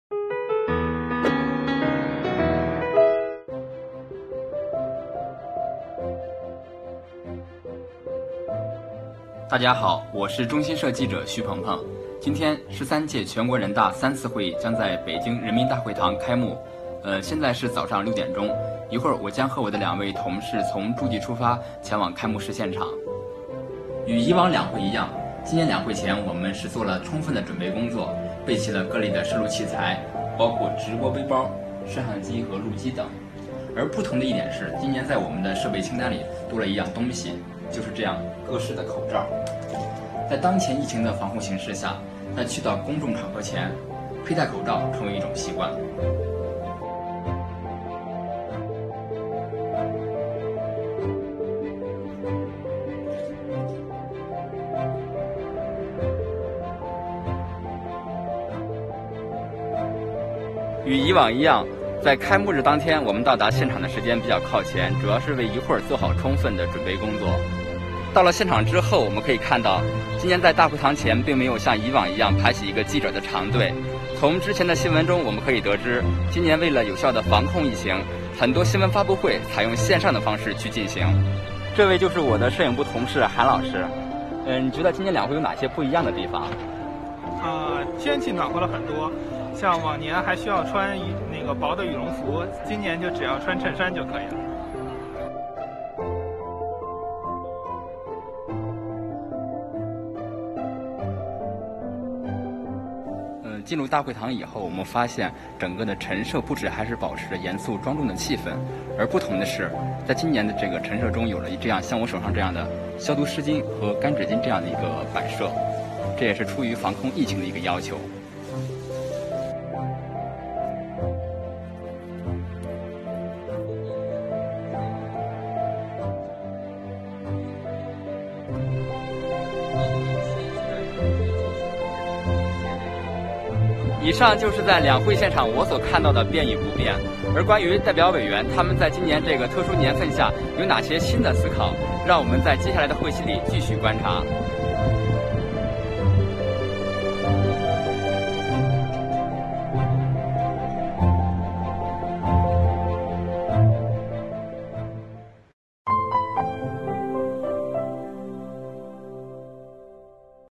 【两会vlog】全国人大会议开幕日：记者眼中的“变与不变”
5月22日，十三届全国人大三次会议在人民大会堂举行开幕会。受新冠肺炎疫情影响，会议推迟了两个多月。疫情形势下，人大开幕会现场有哪些“变与不变”，下面跟着中新社记者的镜头去看一看。